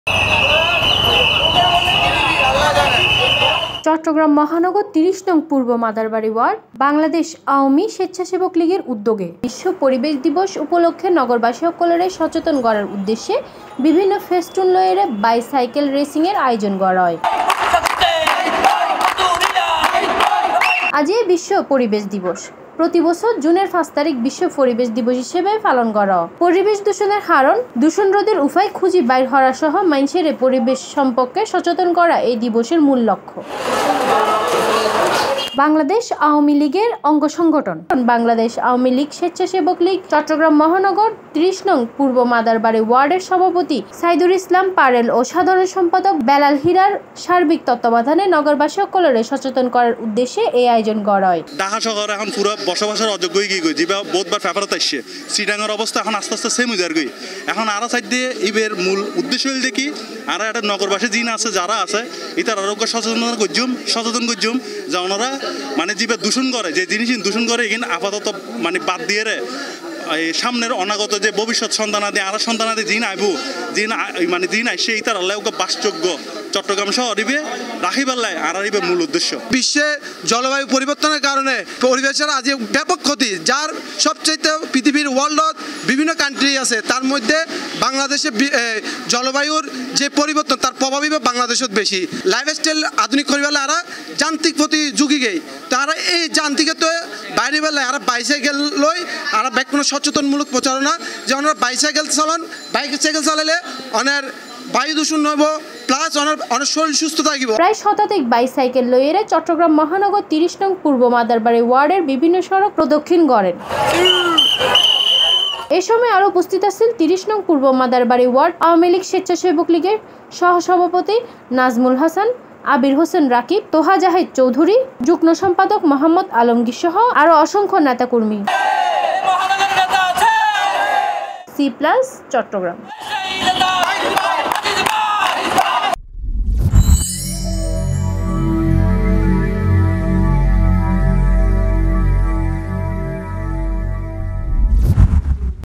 ৩০ নং পূর্ব মাদারবাড়ি ওয়ার্ড আওয়ামী স্বেচ্ছাসেবক লীগের উদ্যোগে বিশ্ব পরিবেশ দিবস পালিত | Cplus
Slogan: আরাঁ চাটগাঁইয়া হতা হই
Country: Bangladesh
Language: Bengali।Chittagong Language
Broadcast area: Online